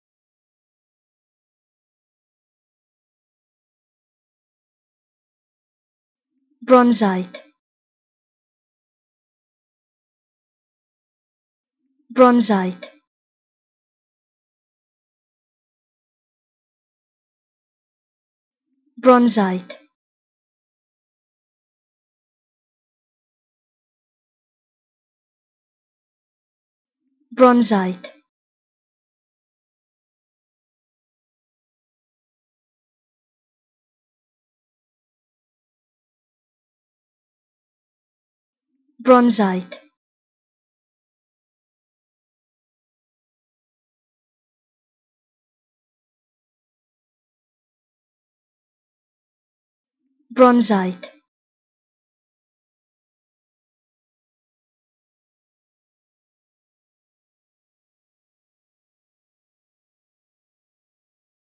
How To Say Bronzite
Learn how to say Bronzite with EmmaSaying free pronunciation tutorials.